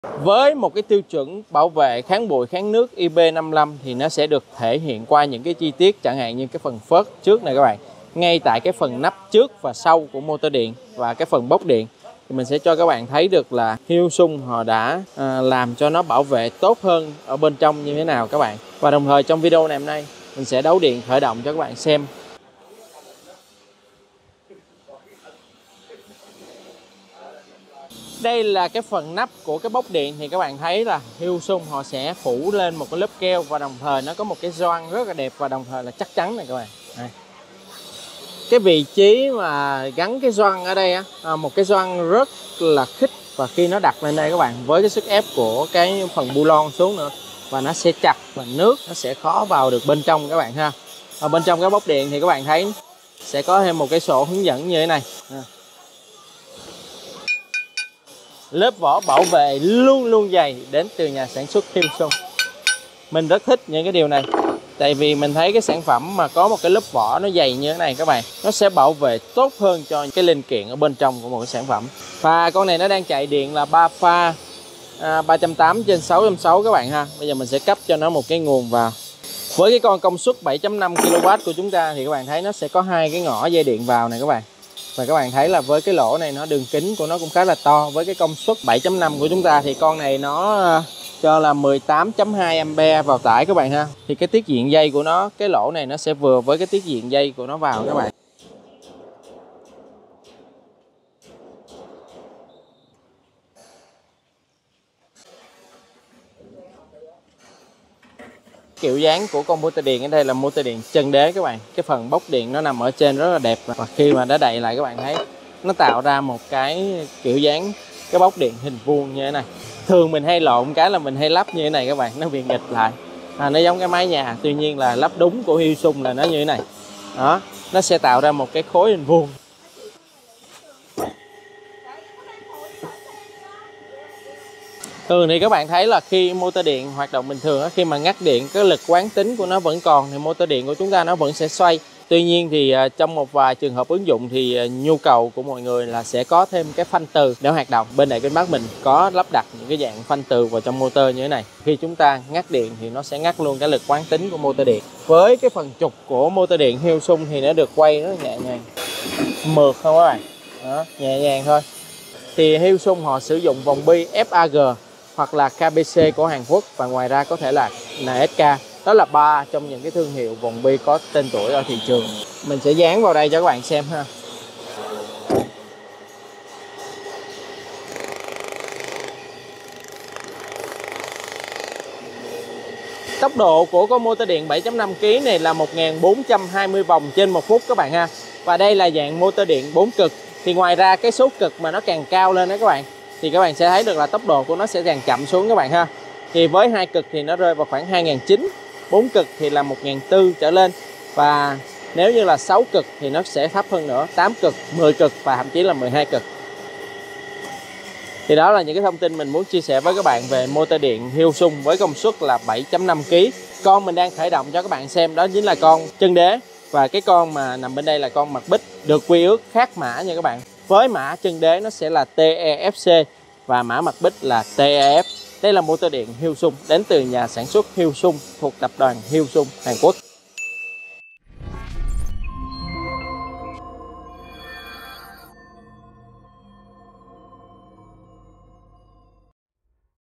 Test motor điện Hyosung 7.5kW chân đế và mặt bích
Trong video này, chúng tôi sẽ giới thiệu về động cơ điện Hyosung công suất 7.5kw chân đế TEFC và mặt bích TEF - một giải pháp động lực hiệu quả cao cho các ứng dụng công nghiệp. Động cơ điện Hyosung được biết đến với độ bền cao, hiệu suất ổn định và khả năng tiết kiệm năng lượng vượt trội. Chúng tôi sẽ không đi sâu vào các chi tiết bên trong sản phẩm. Chúng tôi chỉ nêu các đặc điểm kỹ thuật động cơ điện Hyosung 7.5kw.

Hãy theo dõi để hiểu rõ hơn về cách động cơ điện Hyosung có thể giúp nâng cao hiệu quả hoạt động trong lĩnh vực của bạn, từ sản xuất đến xây dựng và hơn thế nữa.